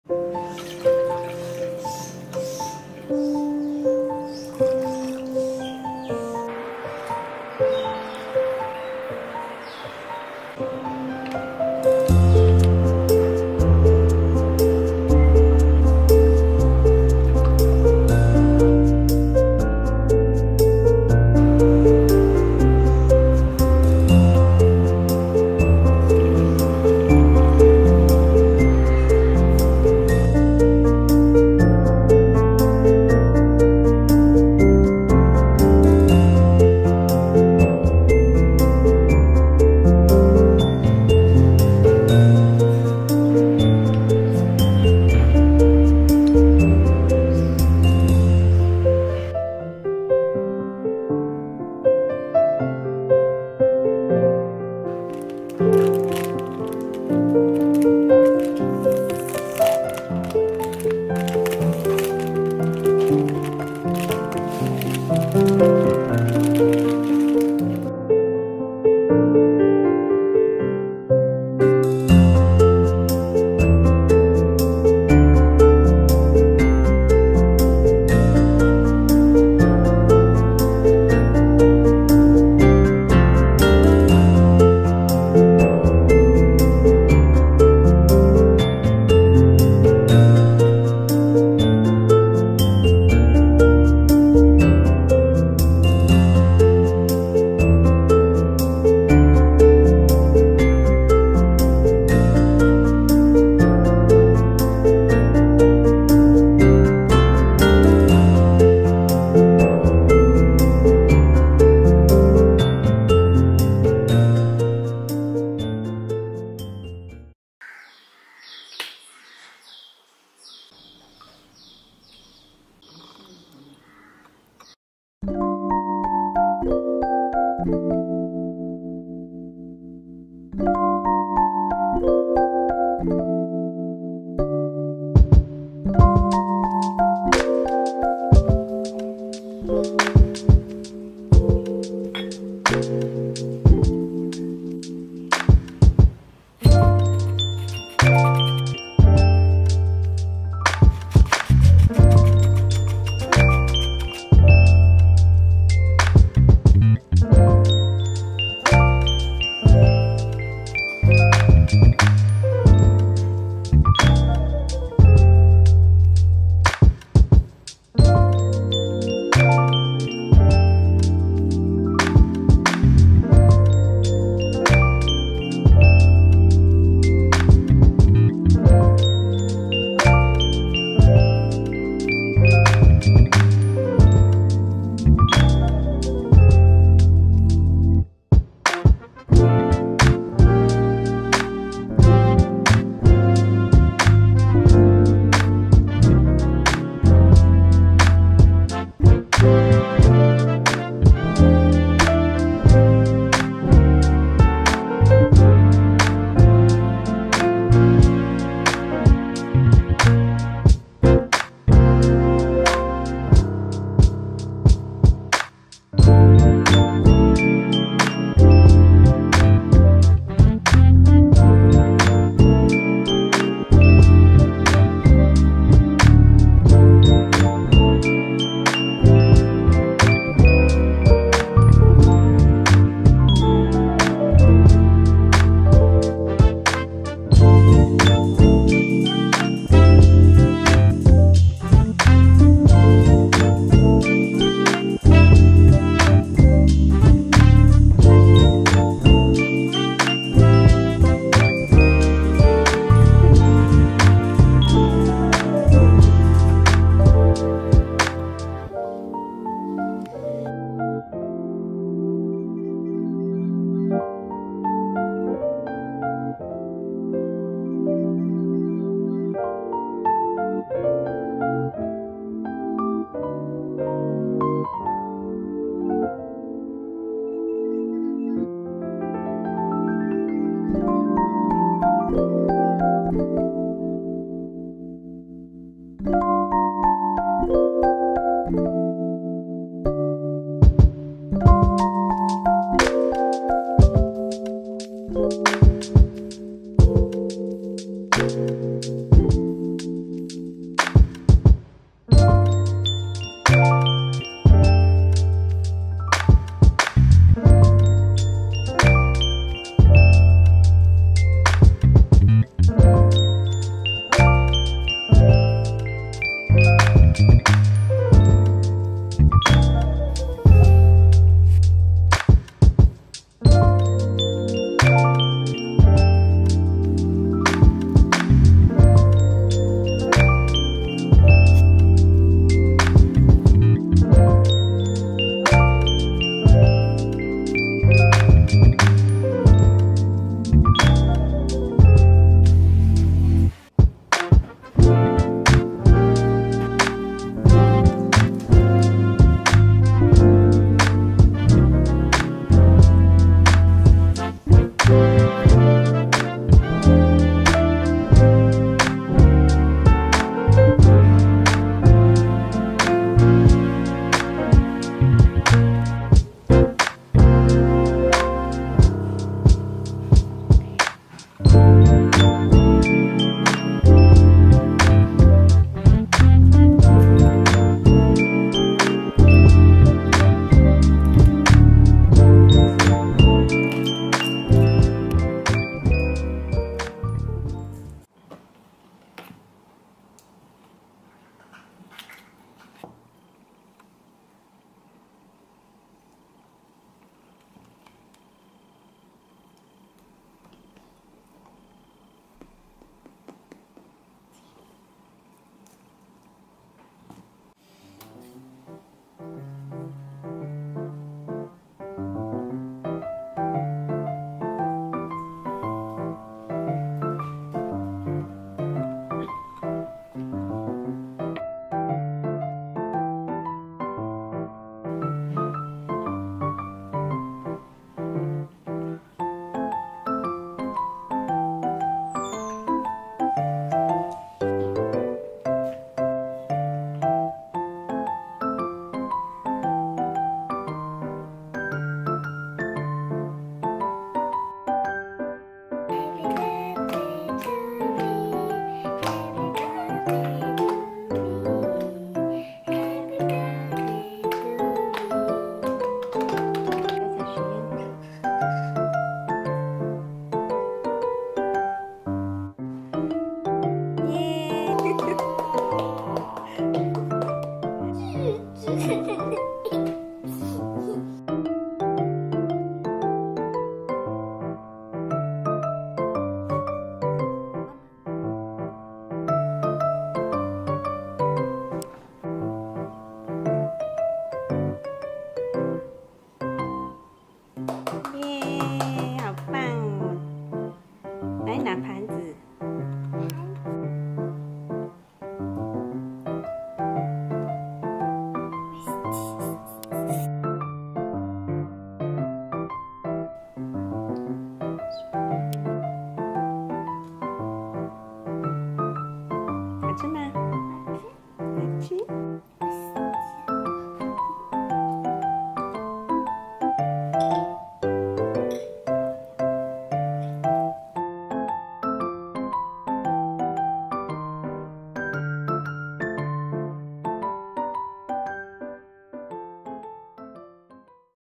温馨简单的女儿生日🥳｜亲手烘焙生日蛋糕和布丁｜A Simple and Warmly of My Daughter’s Birthday 🎂|Handmade Birthday cake
亲爱的朋友们，今天想和大家分享我为女儿庆祝生日的点滴, 我尝试烘焙了我女儿非常喜爱的生日蛋糕和她最爱的布丁, 因为希望她能度过一个难忘的生日🎂也借着这支影片在这里和大家分享我们的喜悦❤️希望大家也有一个美好的一天💕

Cake recipe🎀
蛋  egg   8个
玉米油   Corn  oil  80g
水          Water  80g
低筋面粉  low-gluten flour  140g
砂糖  caster sugar110g

HELLO everyone~ today's video is about the preparation that I've done for my daughter's birthday,  I prepare homemade cake and puddings that she likes the most as I wish she can have a memorable birthday celebration~ Just want to share with you guys the happiness we have that day through this video❤️ Have a good day💕

Music 🎶
Provided by

celebration 생일 케익 생일축하해요 バースデー・ケーキ ハッピーバース・デー